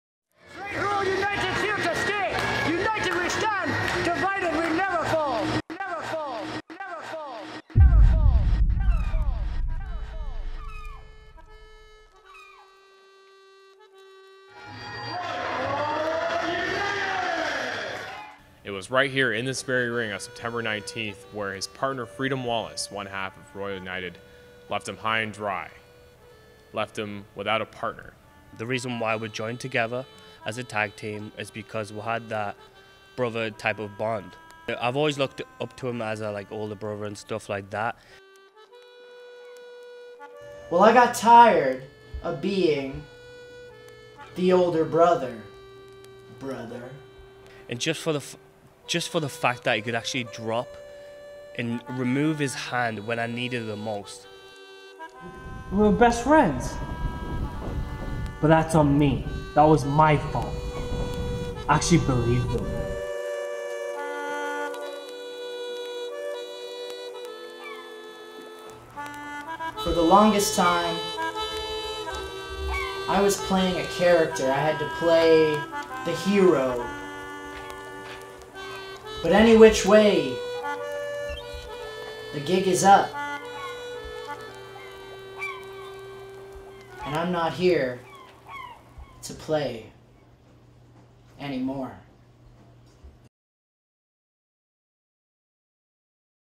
FREEDOM vs. BRAWLER - OFFICIAL MATCH TRAILER
Here's a glimpse at tonight's showdown between FREEDOM and THE BRITISH BRAWLER.  It's been a long time coming...a lot longer than most realize!